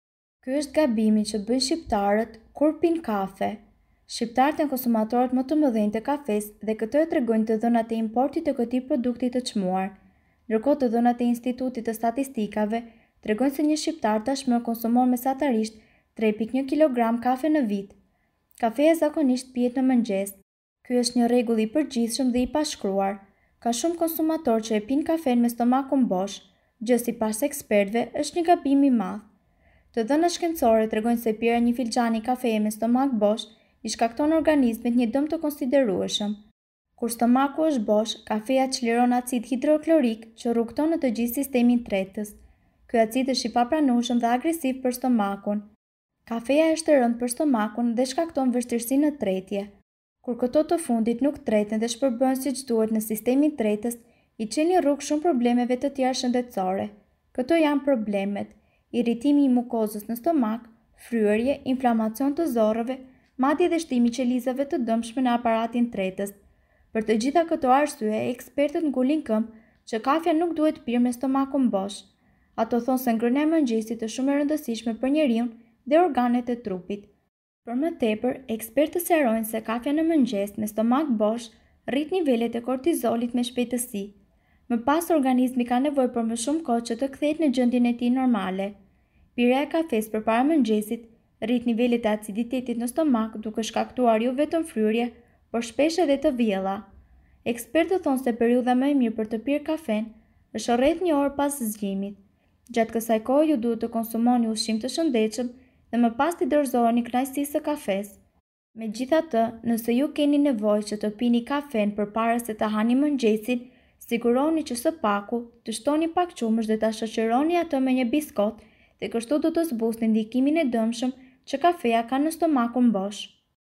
Ky eshte gabimi që bëjnë shqiptarët kur pijnë kafe
Coffee is one of Albanians favorite products. It is consumed en masse in our country. The day does not start without a cup of coffee for an Albanian. Meanwhile, statistics from the Institute of Statistics show that an Albanian now consumes an average of 3.1 kg of coffee a year.